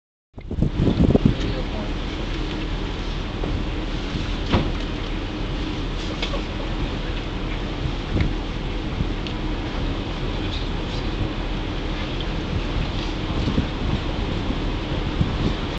Slapende IJsbeer
Deze opname van een slapende ijsbeer heb ik gemaakt op het ijs tijdens mijn Arctica expeditie (juli-augustus 2008) rond positie 79°13' NB - 29°09'OL.